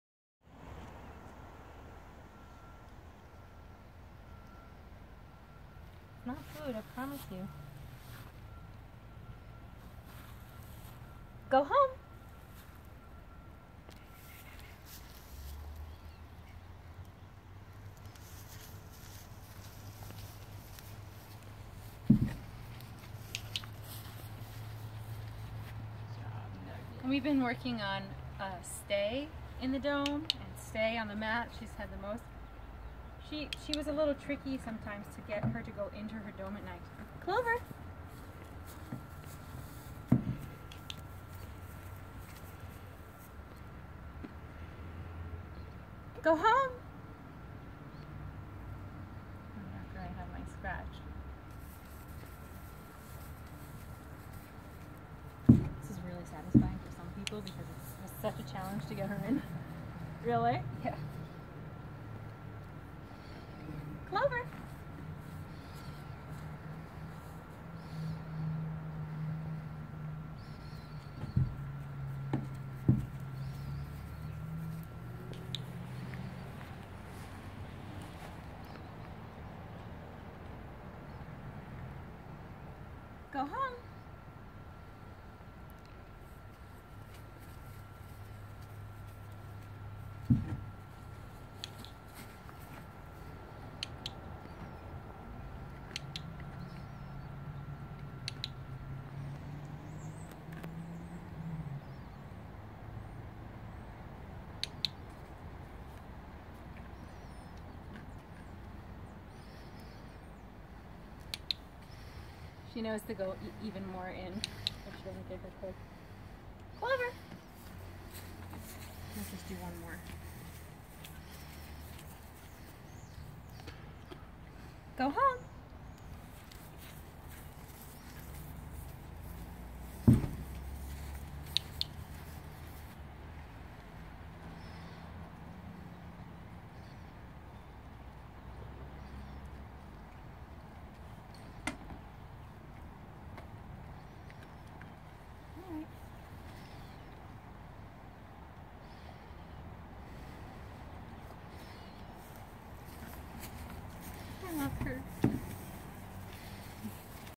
Goat Training - Go Home / go into dome
Session with Clover working on going into her dome on cue
May 2016